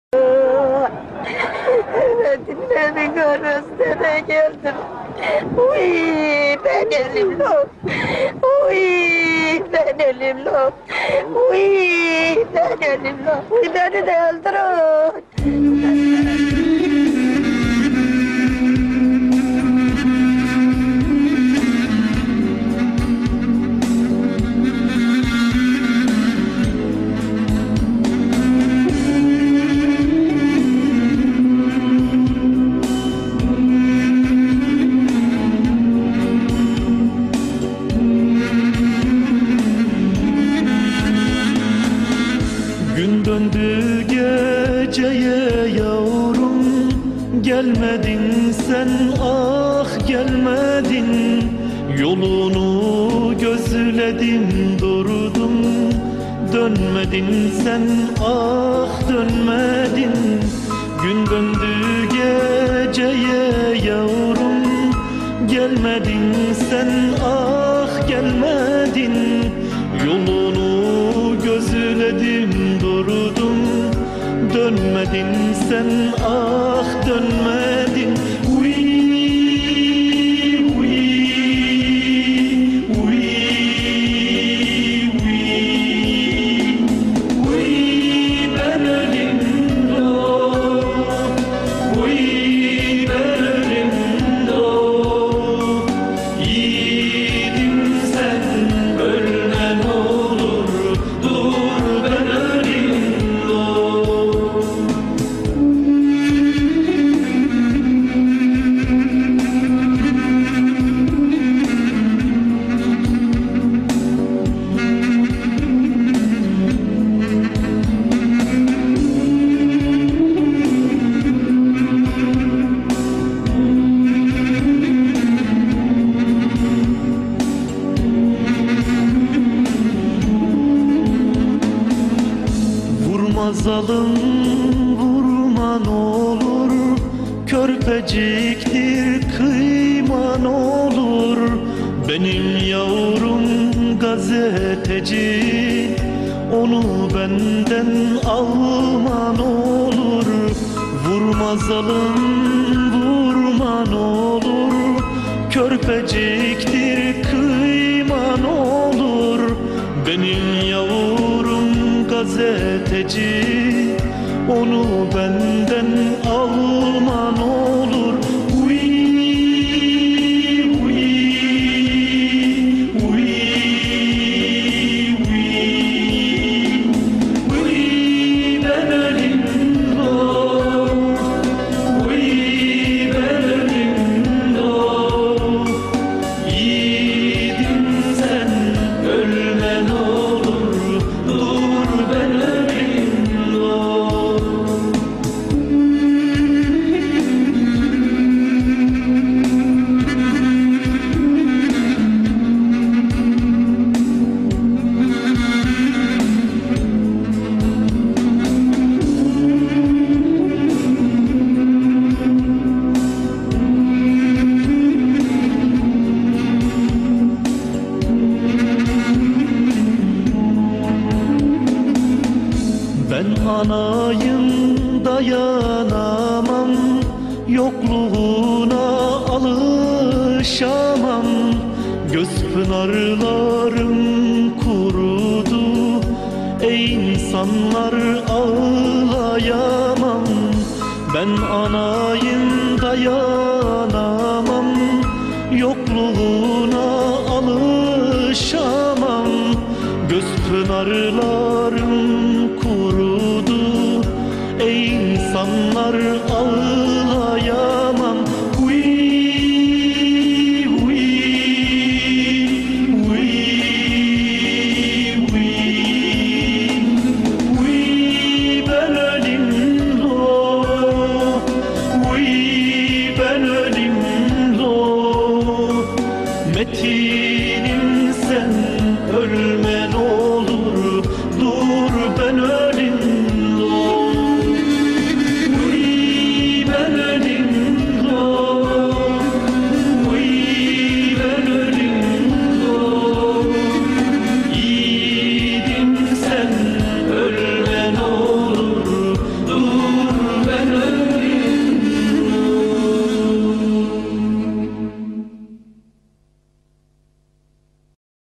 Metin GÖKTEPE
8 Ocak 1996'da İstanbul'da gözaltında işkence yapılarak öldürüldü. (Evrensel gazetesi muhabiriydi)